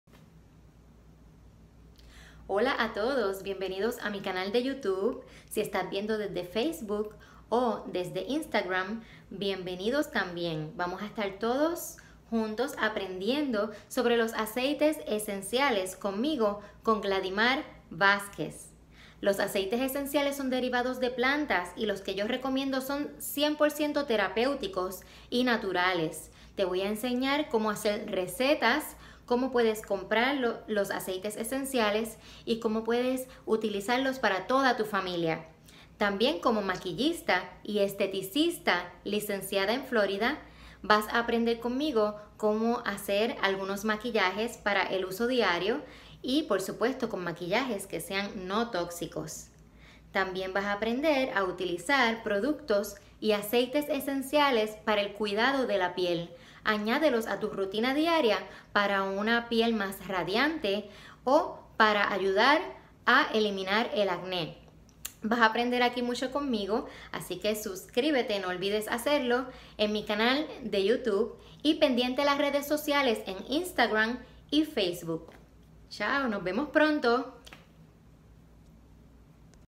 Bienvenidos a mi canal Youtube y Redes sociales.
Les doy la bienvenida a este espacio que es para aprender sobre los aceites esenciales, sus usos y beneficios.